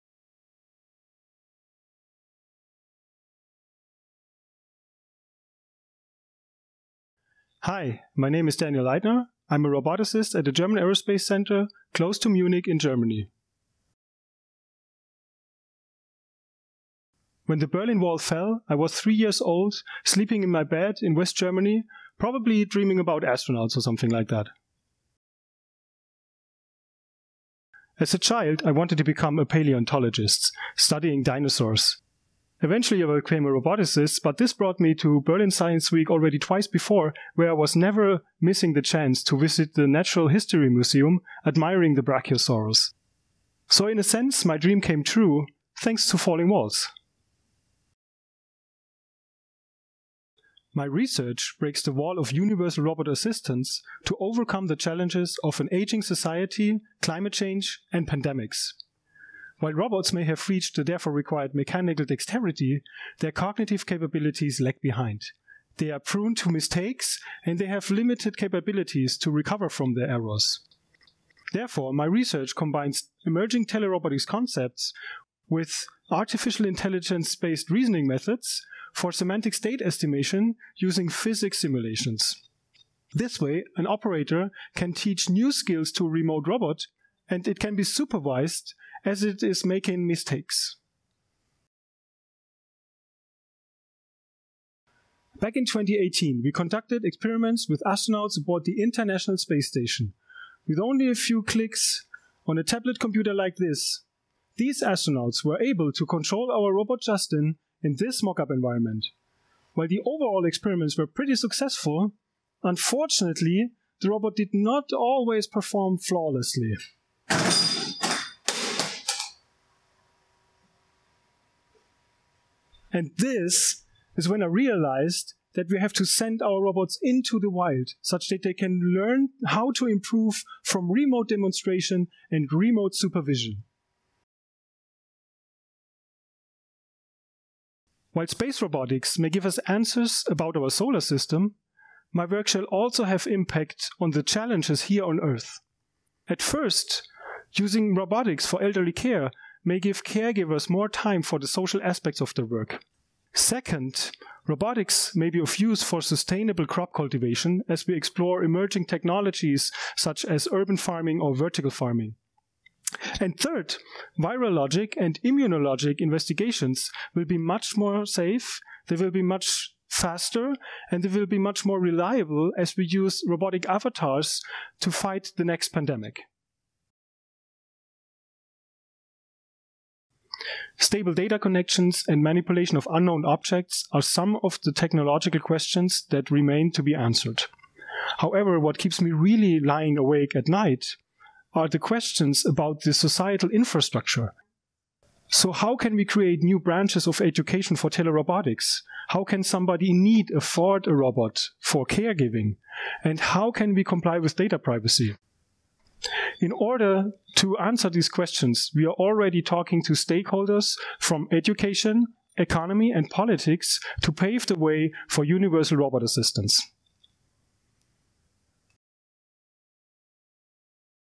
Breaking the Wall of Universal Robot Assistants
Daniel Leidner is a Falling Walls Finalist at the Falling Walls and Berlin Science Week: World Science Summit 2020 (1 – 10 November).

Will he become the Breakthrough of his category Emerging Talent? Register for free and don't miss the remote event